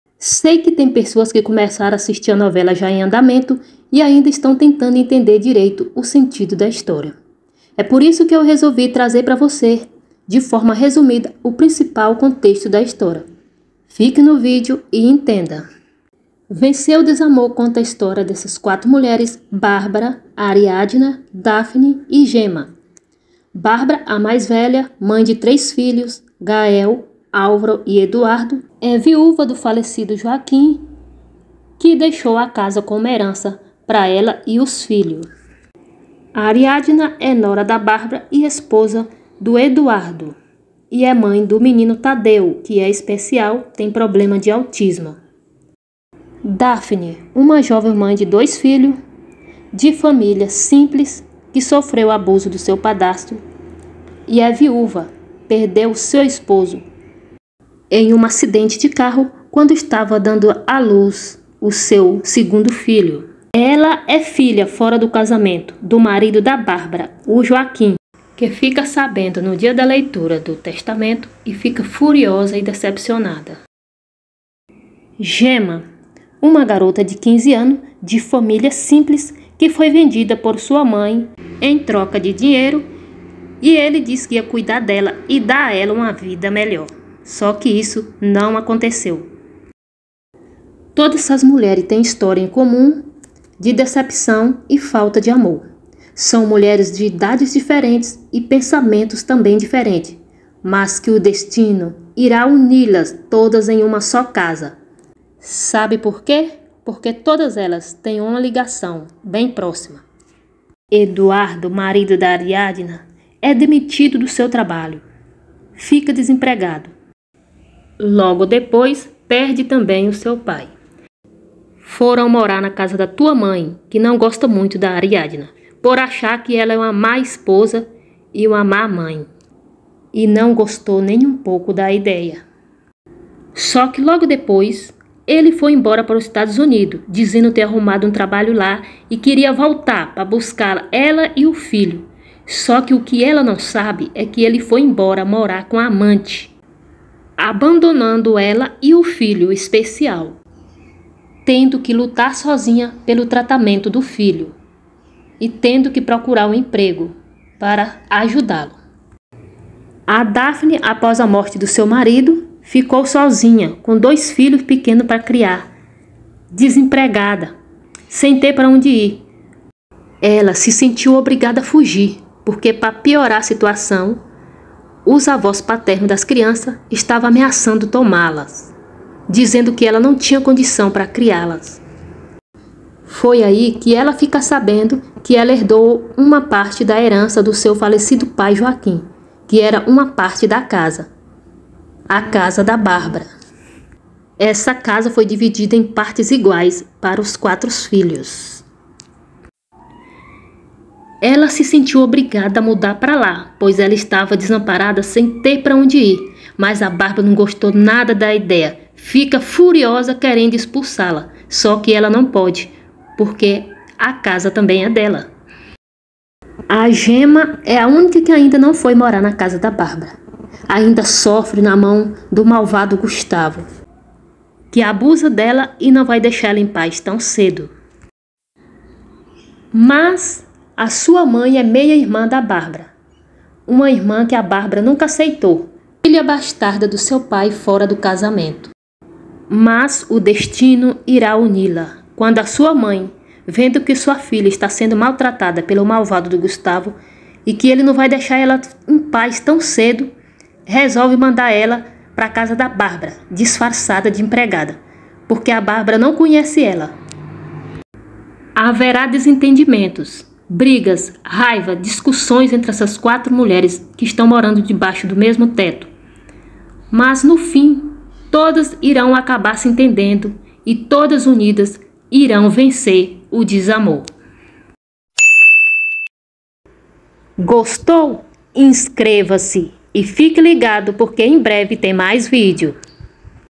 Vencer o desamor/Entenda melhor a história
Como esta novela começou recentemente,resolvi fazer este video porque muitas pessoas disseram nao esta entendendo bem a historia.Vou te explicar de forma resumida o sentido da história da novela vencer o desamor, que fala sobre as  quatro mulheres, que unidas iram enfrentarem os obstáculos e vencerem as decepções da vida. Se você não entendeu bem, assista e entenda .

TVpop.com.br